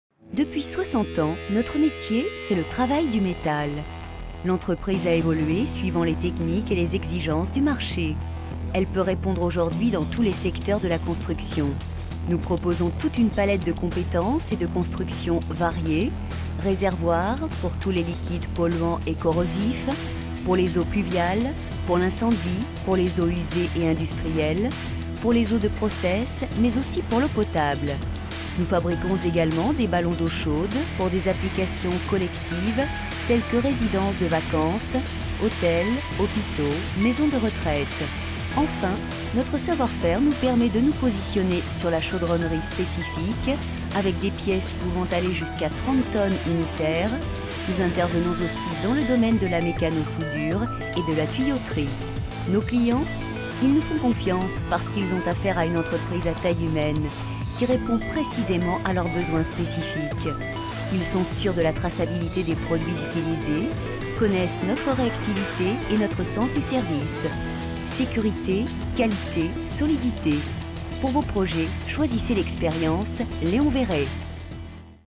Chaudronnerie Léon Veyret à Romans-sur-Isère dans la Drôme (26)
- Les établissements Leon Veyret, chaudronnerie spécialisée depuis un demi-siècle dans la fabrication de réservoirs de stockage toutes capacités et tous fluides.

Acteur majeur dans la production d'eau chaude sanitaire et le stockage des hydrocarbures, nous sommes aussi présents dans le domaine environnemental, la rétention et régulation des eaux pluviales, les bassins d'orage ainsi que la fabrication de réserves incendies répondant aux exigences imposées par les SDIS.

Notre outil de production, neuf et ultra-moderne, ainsi que notre équipe expérimentée et motivée, seront à même de trouver une solution à tous vos besoins.